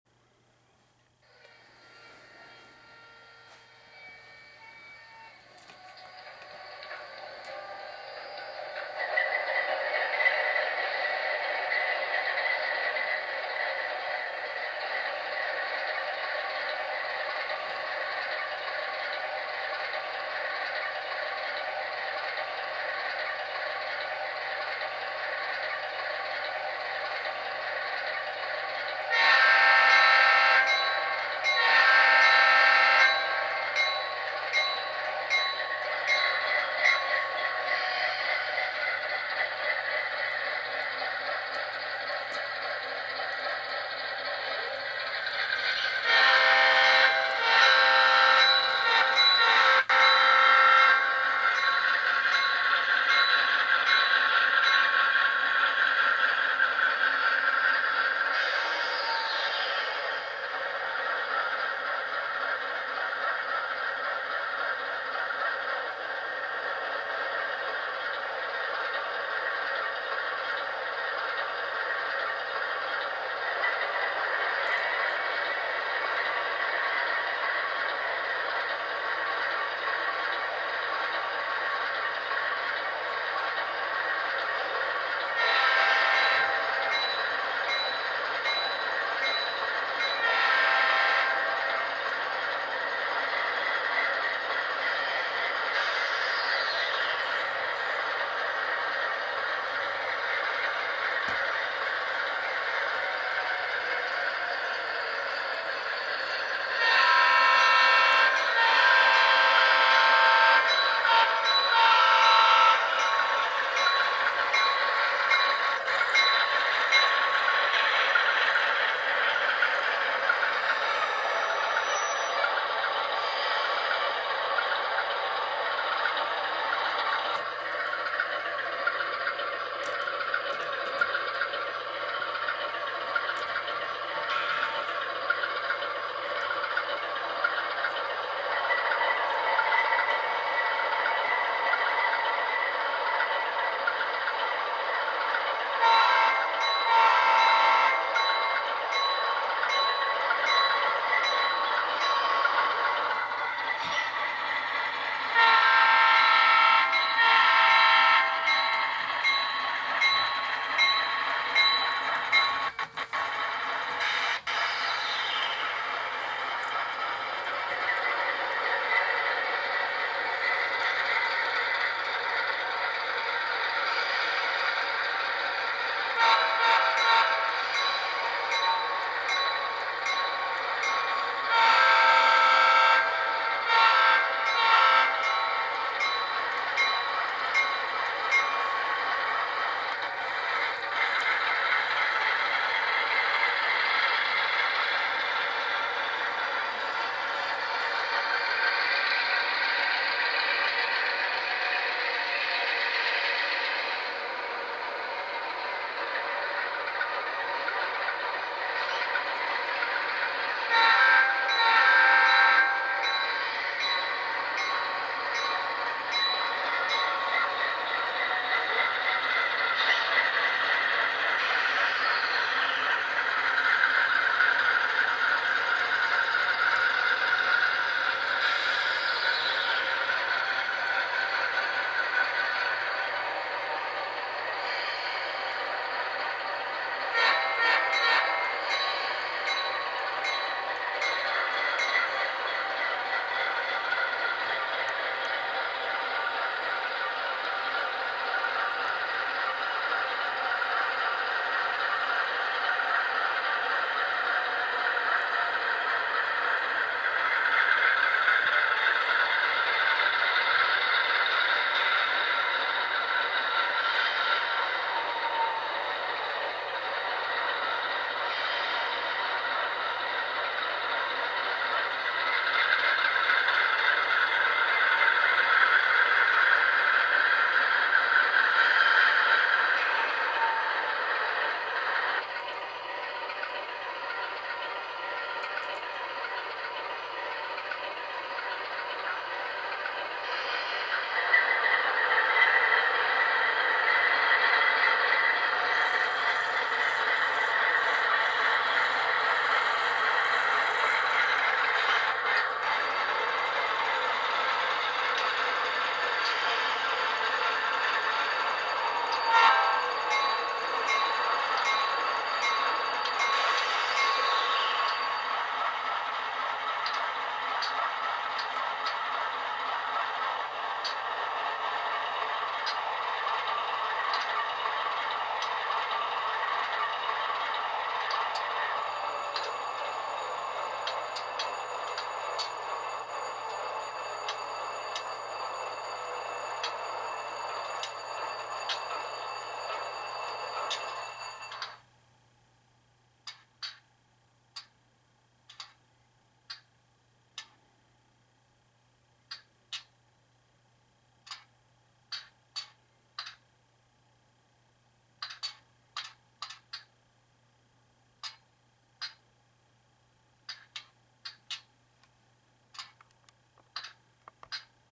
Santa Fe C44-9W #612 and #617
Santa Fe C44-9W "Dash-9" pair #612 and #617 equipped with ESU LokSound Select Direct decoders and a pair of iPhone 5 speakers and LED lighting. Models by Kato.